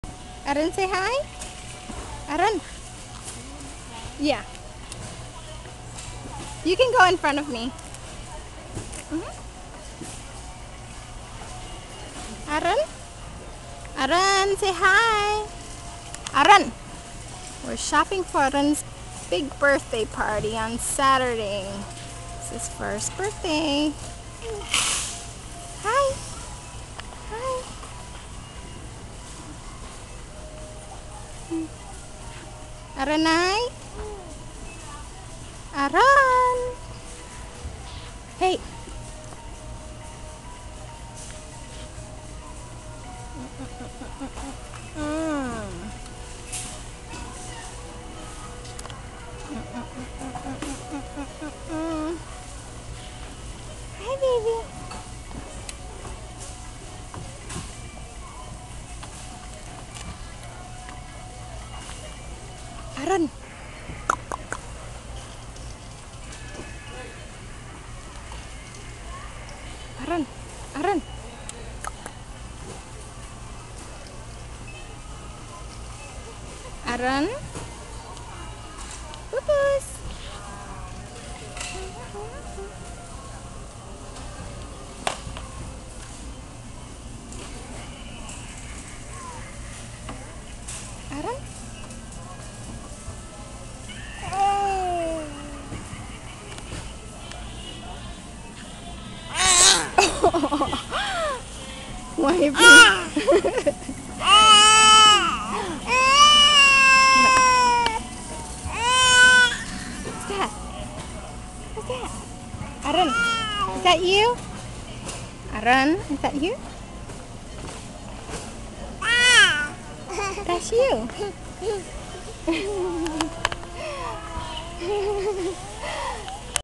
At Costco
Shopping for food and supplies for Ahrun's 1st birthday party. 8/24/08